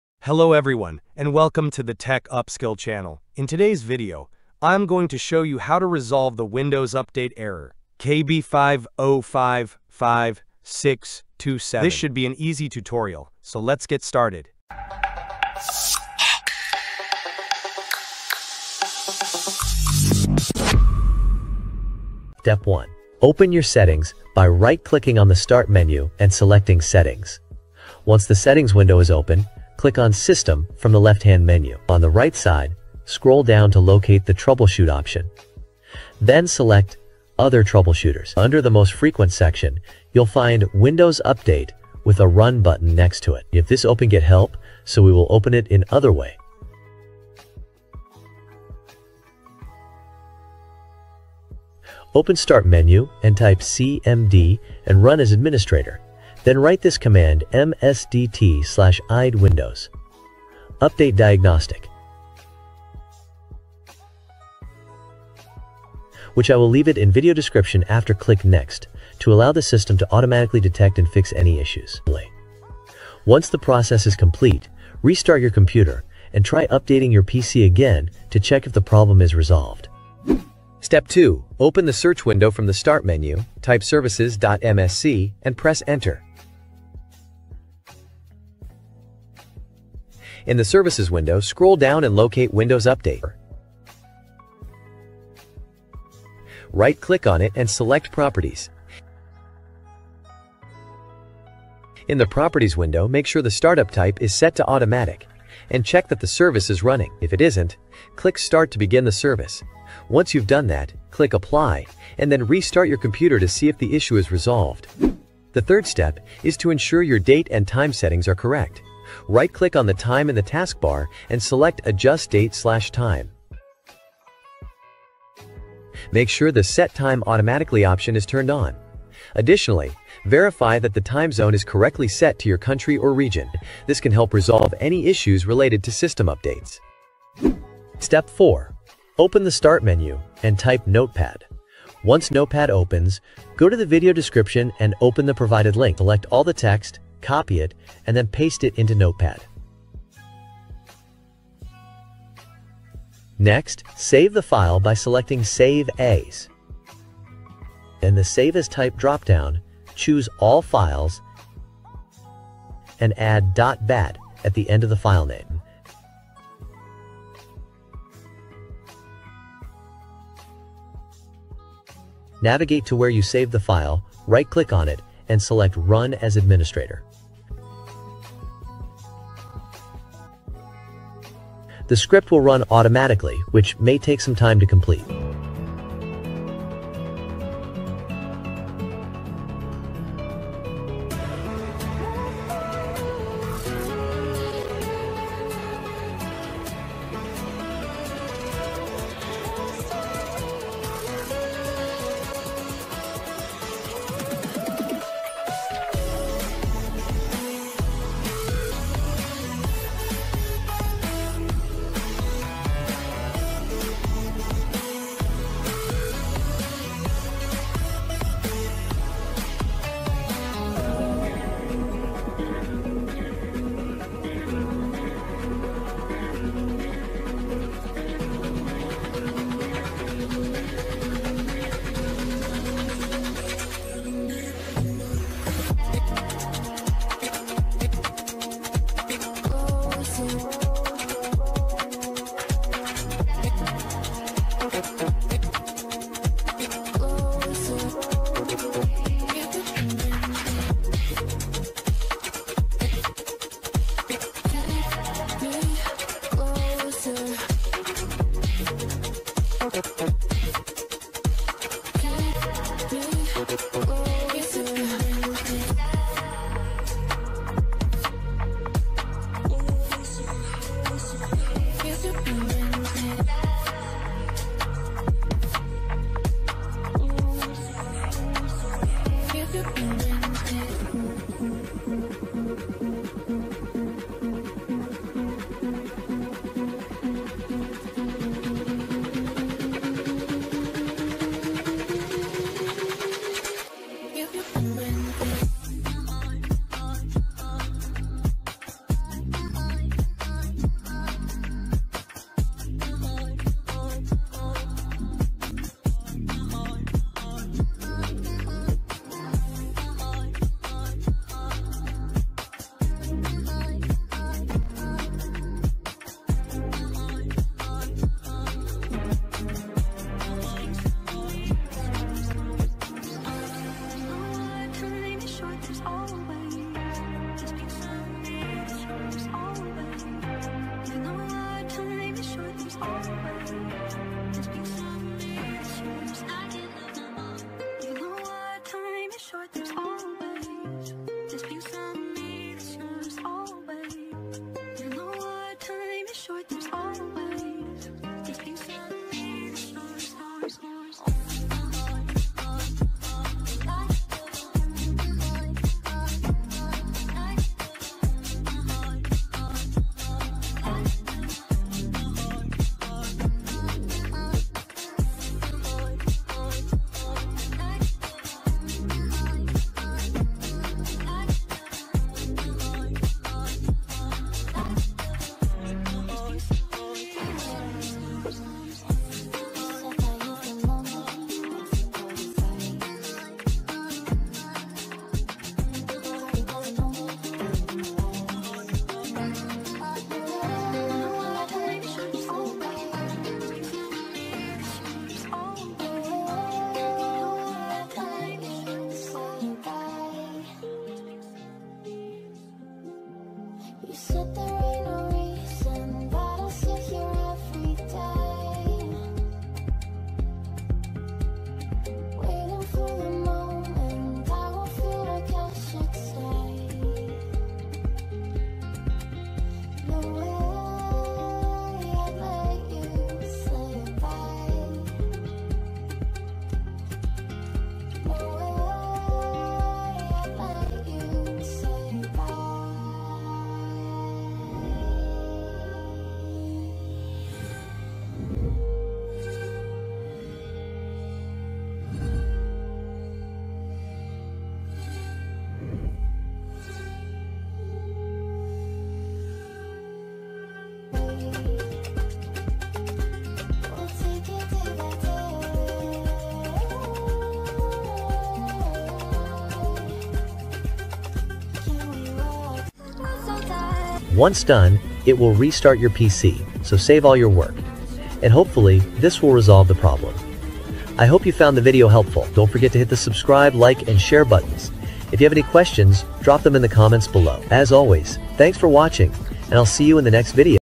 [Solved] ✔️ Windows Update KB5055627 Failure ⚠️ Fix Update KB5055629 Fails Windows
In this video, learn how to fix Windows Update Error Code  KB5055627  installation failed issues with Windows 11 Update Not Installing Install Error KB5055629 On Windows 10 Fix Windows Update Error KB5055627

⏱️ Timestamps ⏱️
00:00 Intro
00:22 Step 1
01:19 Step 2
01:54 Step 3
02:17 Step 4